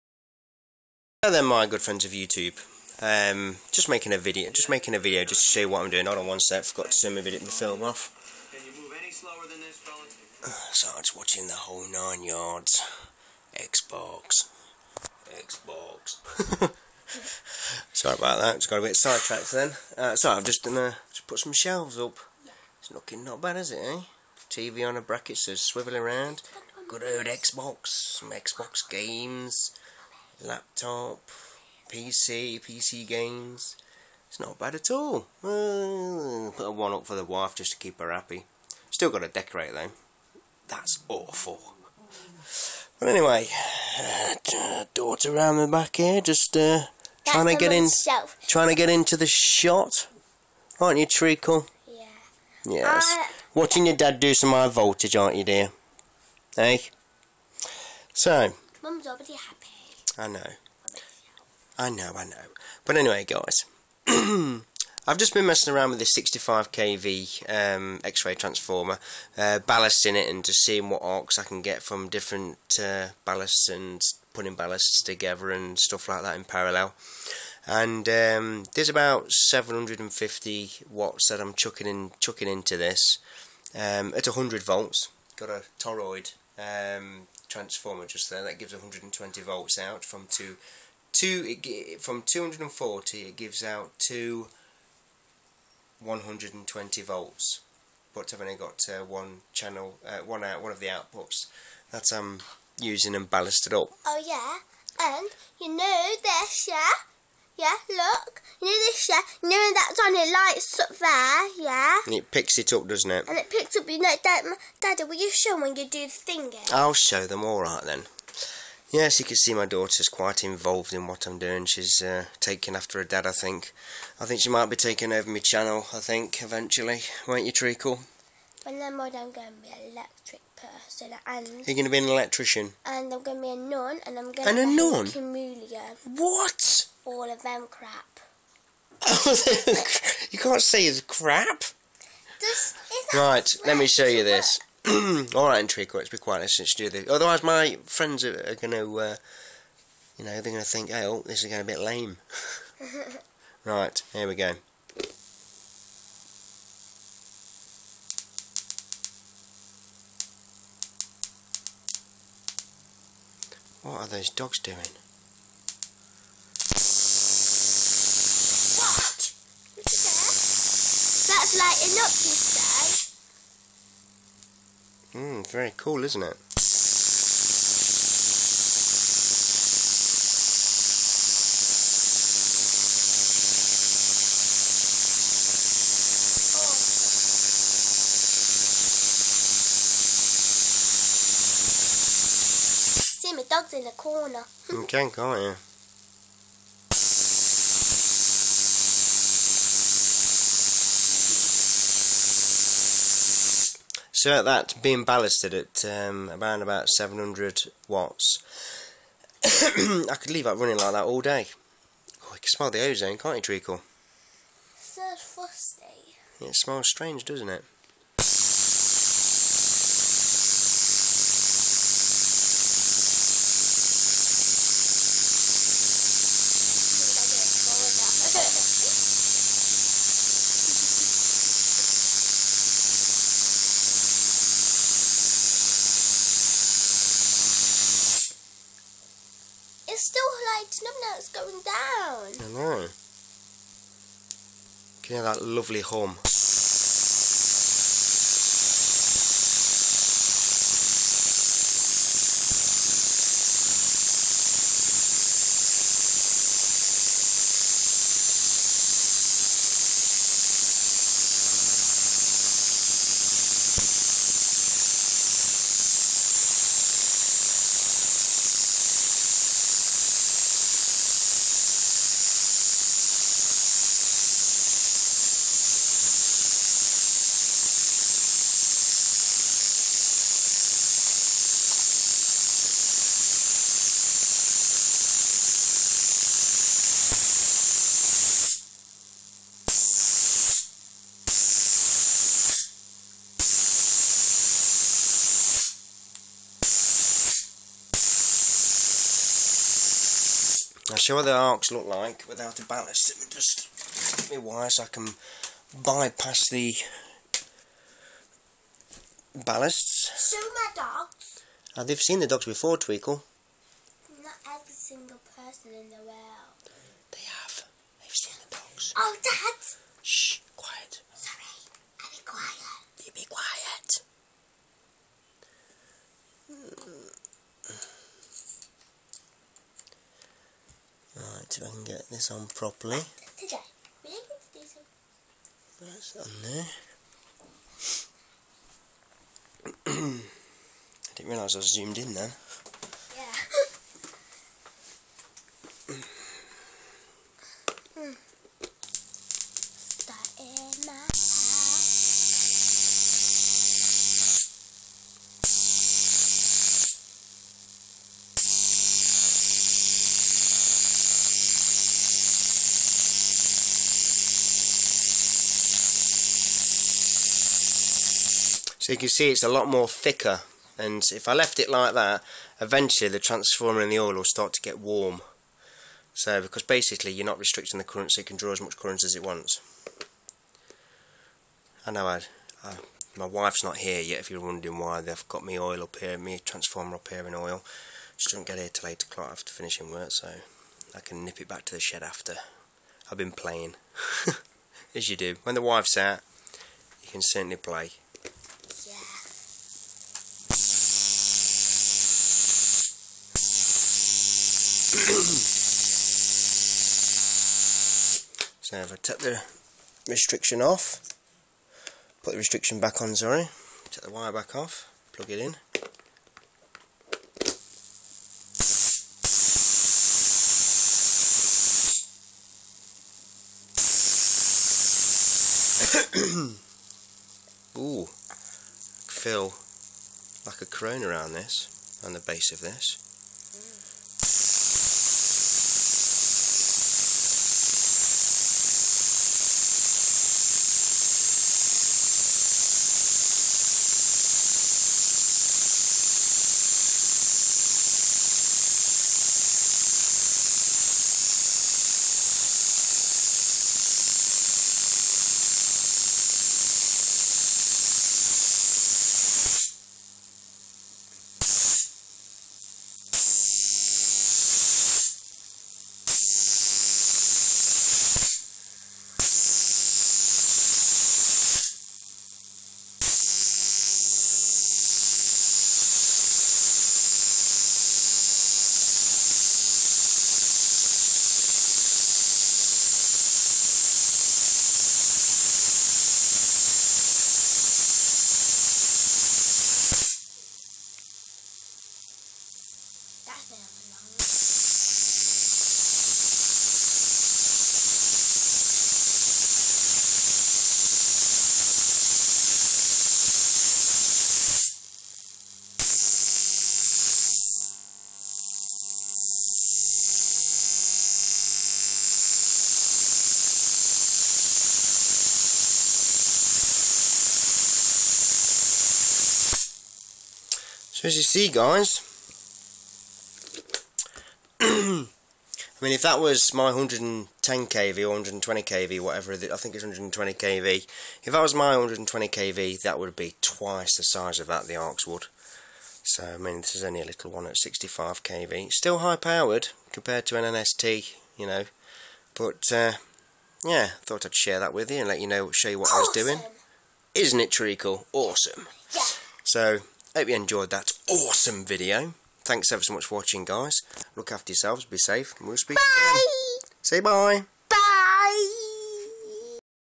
Ballasting a x-ray transformer
Having my time playing with high voltage x-ray transformer ballasting it to see what I can get out of it! At 120 volts. To no damage it! lol
Hope ya like me video and thanx for watching friends ;-)